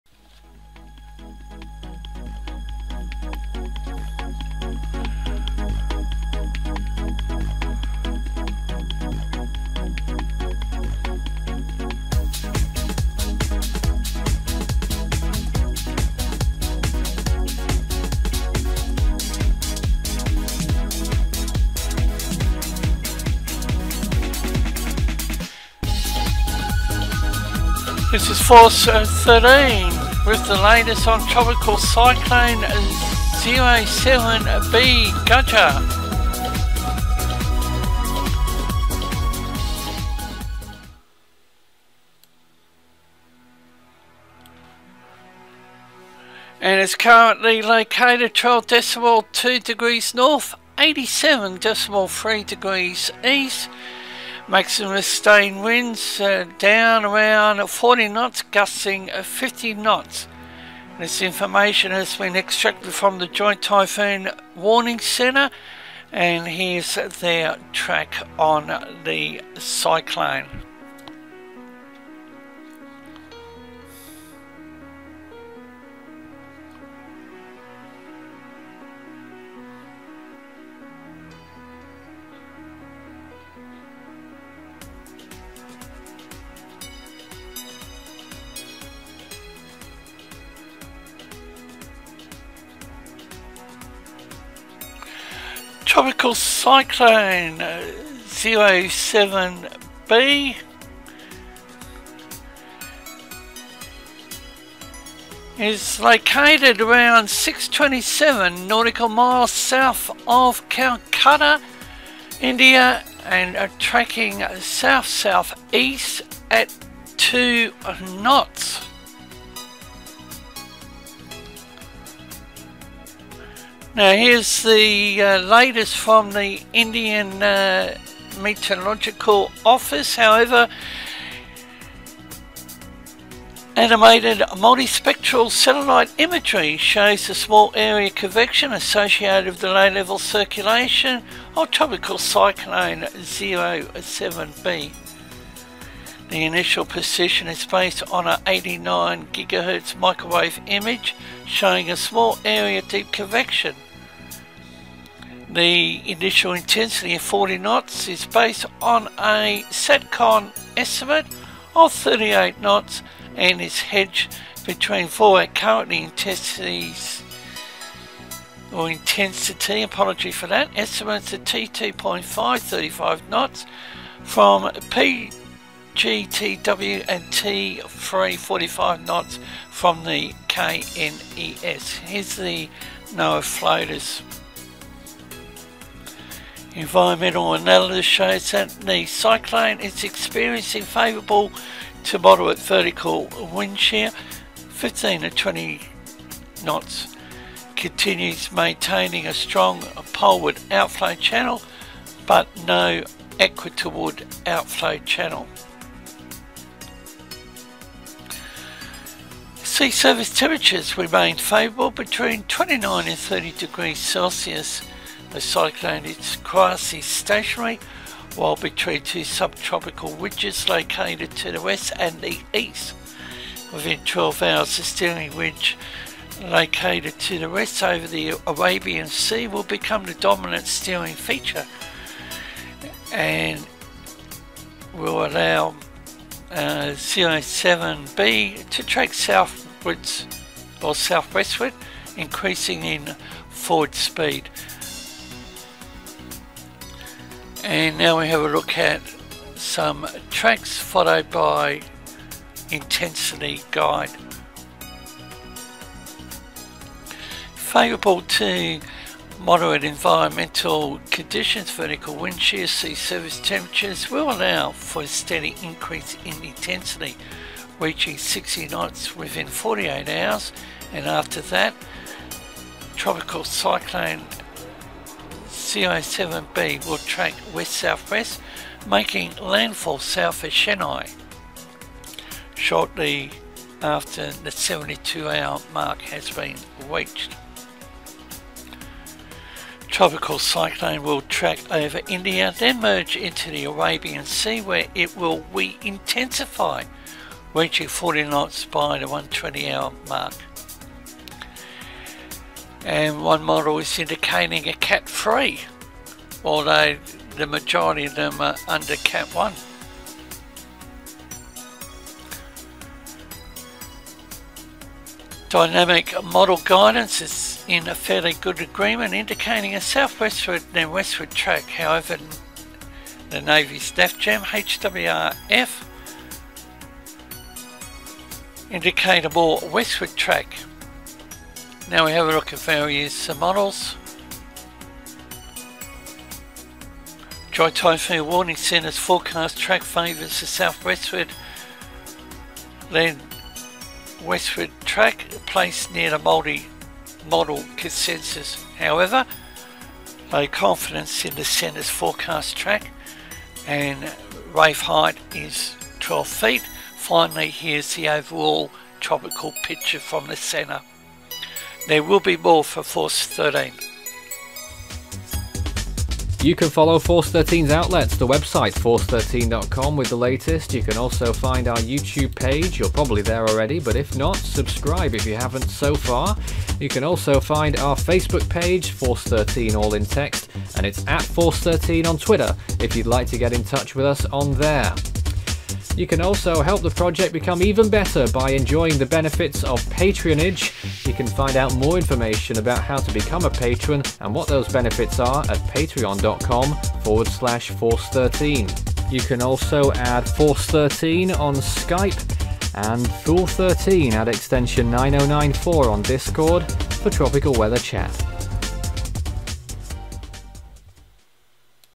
India Tropical Cyclone( 07B) Gaja November 12 India Time - 15:06Hrs
WARNING POSITION:
   120600Z --- NEAR 12.2N 87.3E
     MOVEMENT PAST SIX HOURS - 155 DEGREES AT 02 KTS
     POSITION ACCURATE TO WITHIN 040 NM
     POSITION BASED ON CENTER LOCATED BY SATELLITE
   PRESENT WIND DISTRIBUTION:
   MAX SUSTAINED WINDS - 040 KT, GUSTS 050 KT

TROPICAL CYCLONE (TC) 07B (GAJA), LOCATED APPROXIMATELY 627 NM SOUTH OF CALCUTTA, INDIA, HAS TRACKED SOUTH-SOUTHEASTWARD AT 02 KNOTS OVER THE PAST SIX HOURS. ANIMATED MULTISPECTRAL SATELLITE IMAGERY SHOWS A SMALL AREA OF CONVECTION ASSOCIATED WITH THE LOW LEVEL CIRCULATION CENTER (LLCC) OF TC 07B.